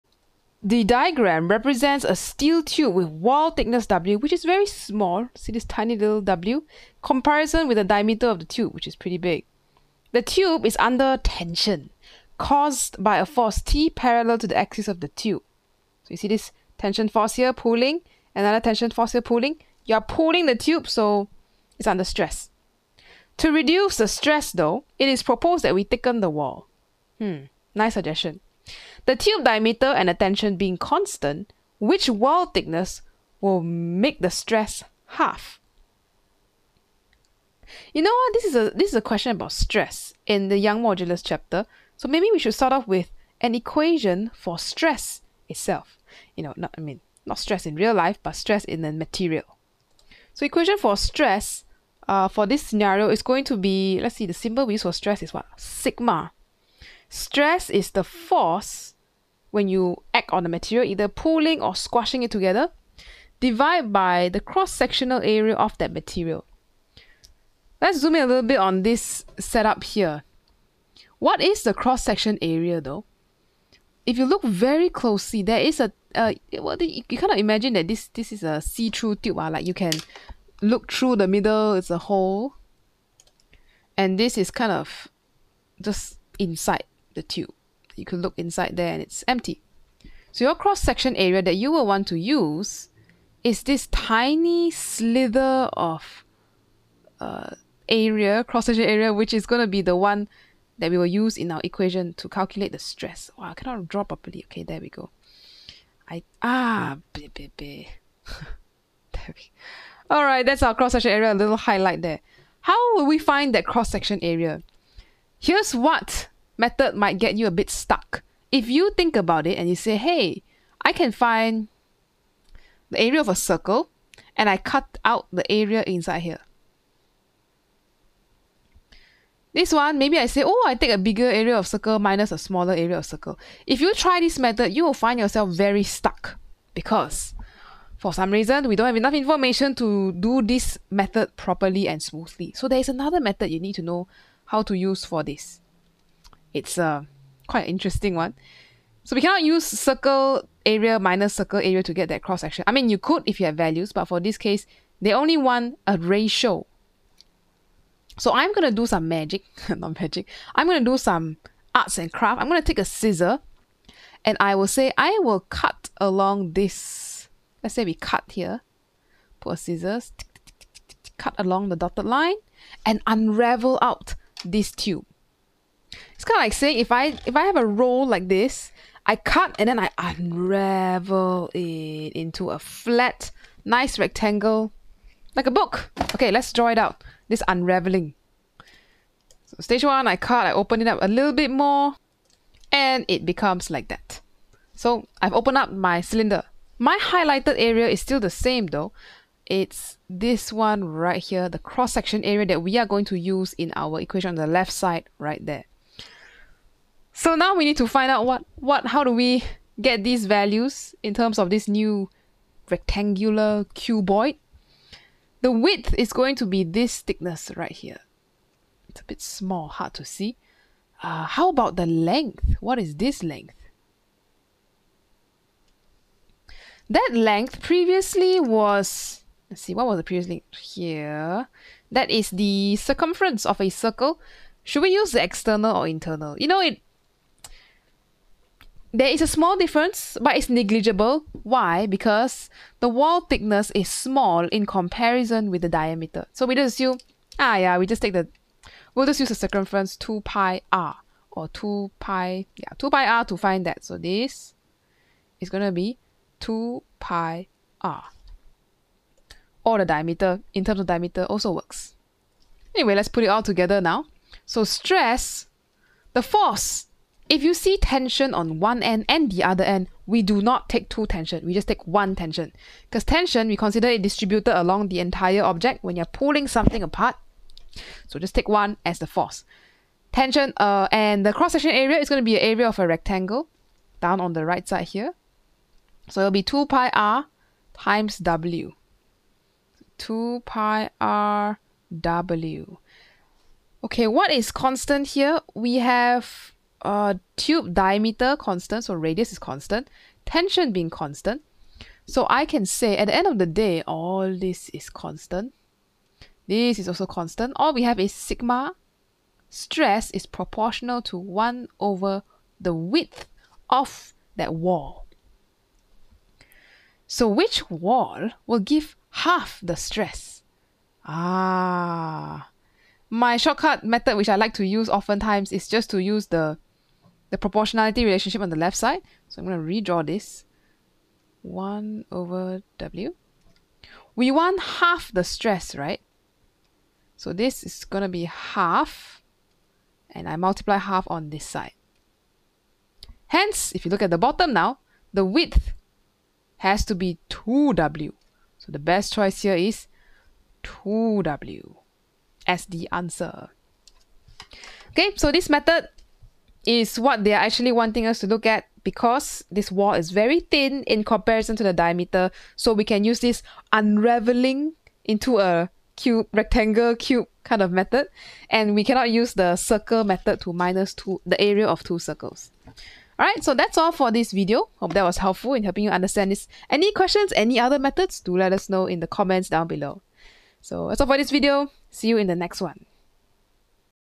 6.1a Ex4 MJ15 P12 Q23 Tube Wall Thickness Stress | May/June 2015 | Cambridge A Level Physics
Example 4 - 9702/12/M/J/15: The diagram represents a steel tube with wall thickness w which is small in comparison with the diameter of the tube. The tube is under tension, caused by a force T, parallel to the axis of the tube. To reduce the stress in the material of the tube, it is proposed to thicken the wall. The tube diameter and the tension being constant, which wall thickness gives half the stress?

9702_s15_qp12